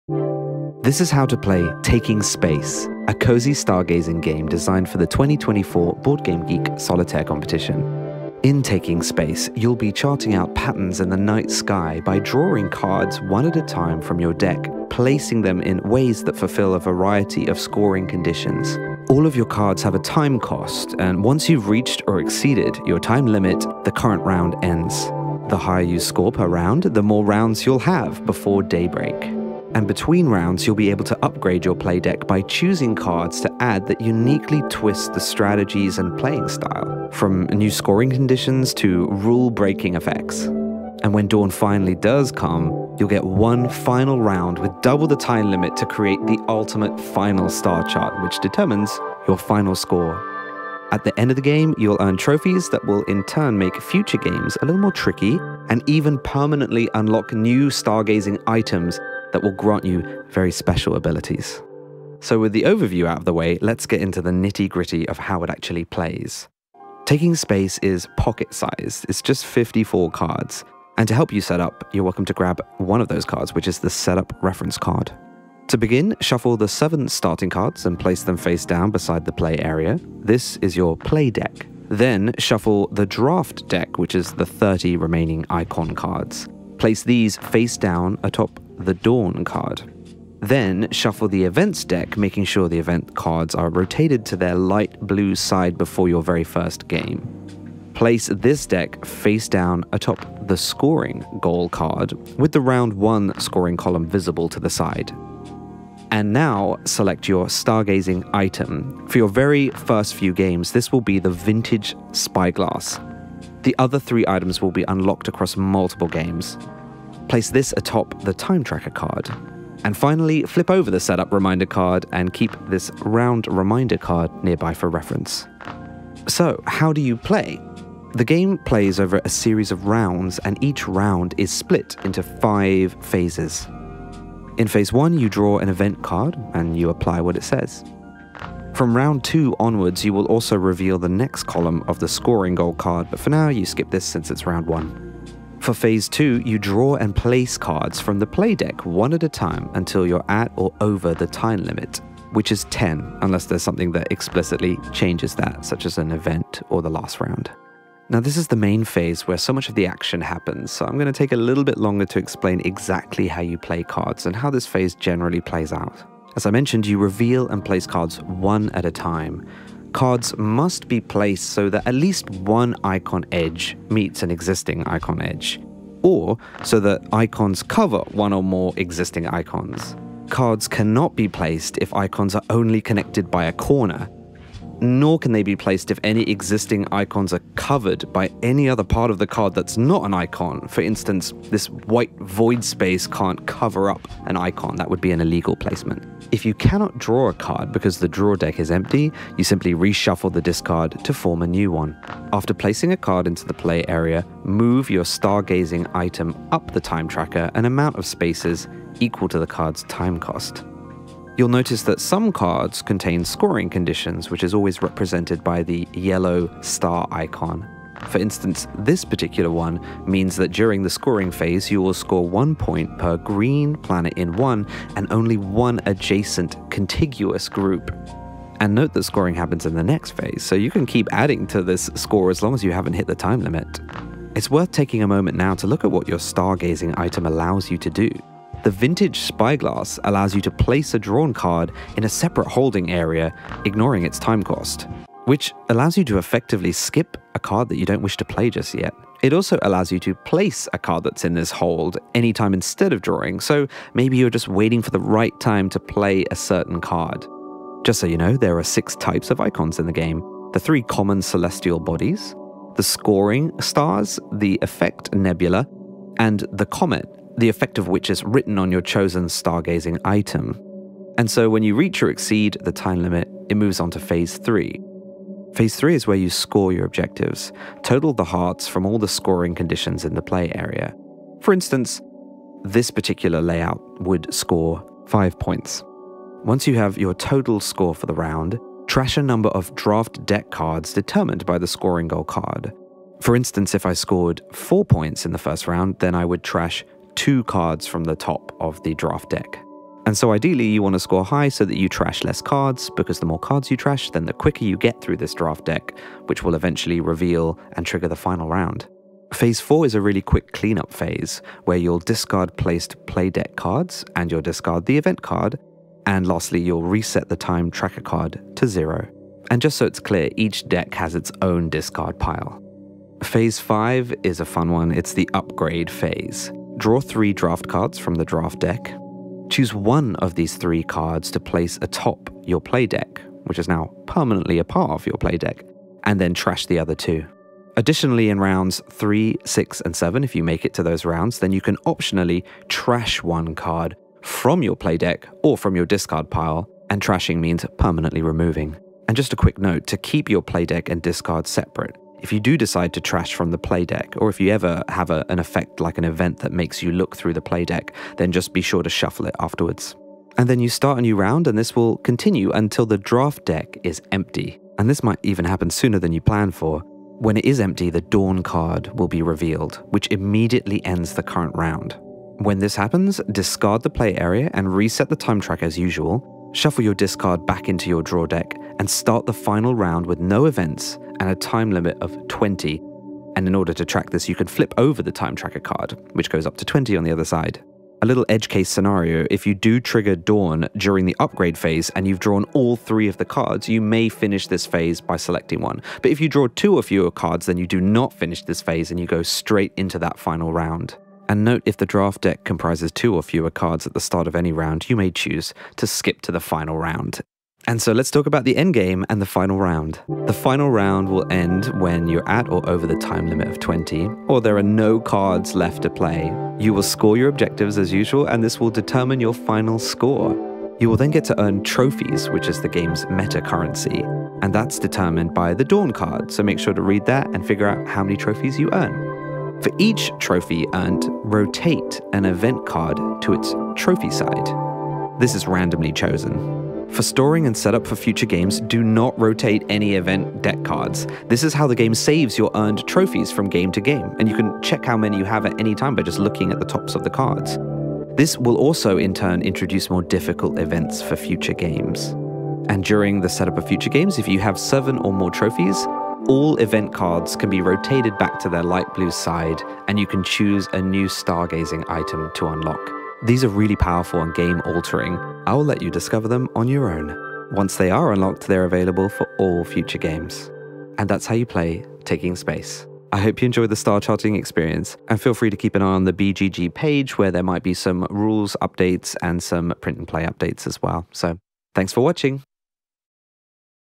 TAKING SPACE - How To Play (solo pattern-laying deckbuilder game)
Taking Space is a cozy pattern-laying deckbuilder, designed for the 2024 BoardGameGeek Solitaire competition! Chart out patterns in the night sky in this pocket-sized card game for 1 player (54 cards total). Designed by Brave James, art by Jon Tiburzi.

00:00 Overview
01:02 Setup
02:06 How To Play
02:14 Phase 1
02:25 Phase 2
04:35 Phase 3
05:09 Phase 4
05:24 Phase 5
06:00 Final Round
06:47 Endgame
0:750 Outro